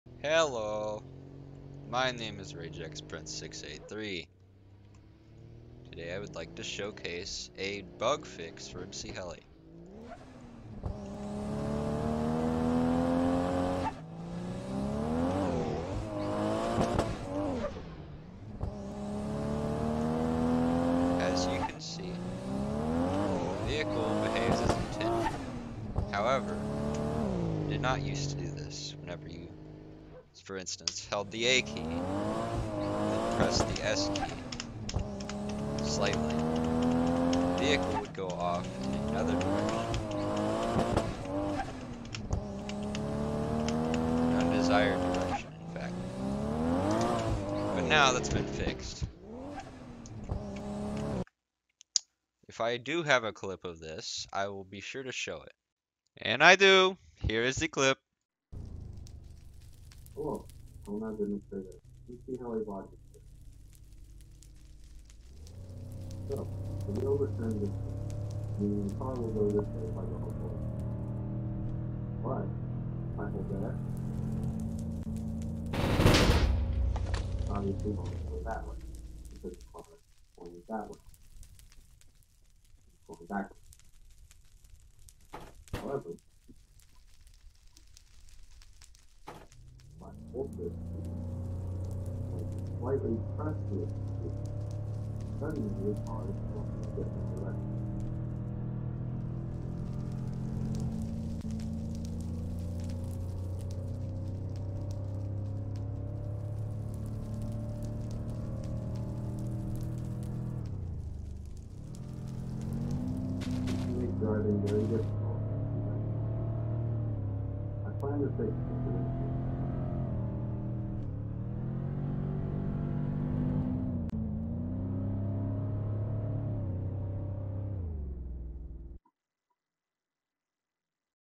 mchelio devlog: 2
'I can't turn' the bug has been semi fixed. Could definitely still be polished though

mchelicopter on top tho fr